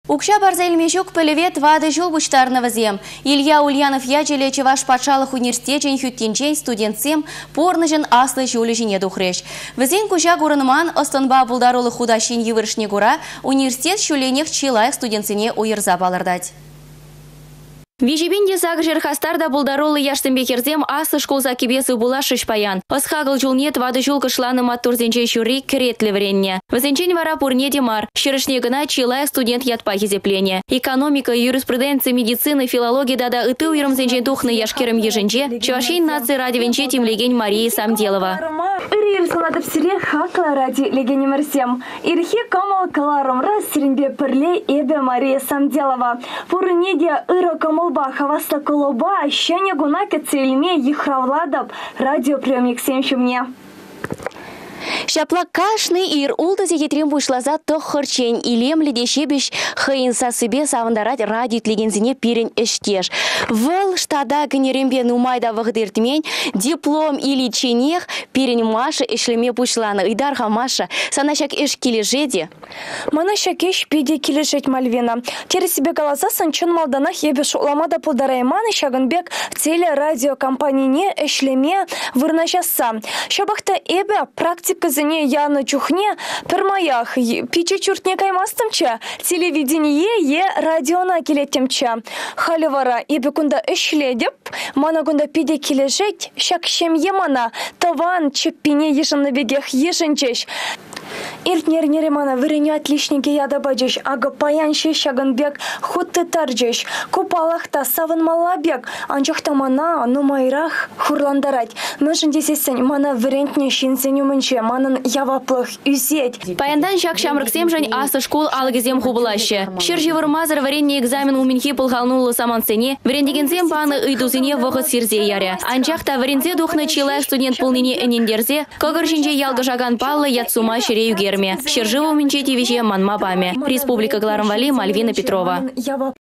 ЧПУ чи лайăх студентсене палăртрĕ
Укçа парса илме çук пĕлĕве 4 çул пуçтарнă вĕсем. Илья Ульянов ячĕллĕ Чăваш патшалăх университечĕн хÿттинчен студентсем пурнăçăн аслă çулĕ çине тухрĕç. Вĕсен куçа курăнман ăс-тăнпа пултарулăх хутаççин йывăрăшне кура университет çулленех чи лайăх студентсене уйăрса палăртать.